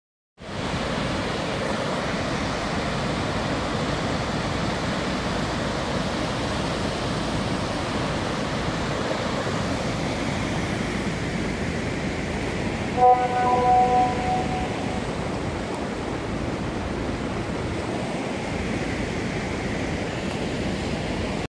Torrent de Canyamel 19-12-2016
Així baixa l'aigua després de les plogudes.